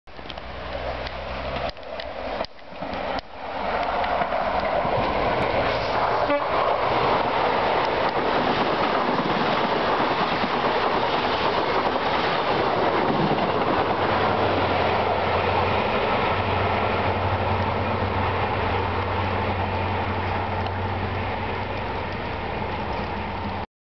HST Fast through Par - Part 1
FGW Class 43's no's 43163+43190 tank it through Par with a service to Penzance. Thanks to the driver for the horn, and to BRsteam70013 for the company. Ceck out his channel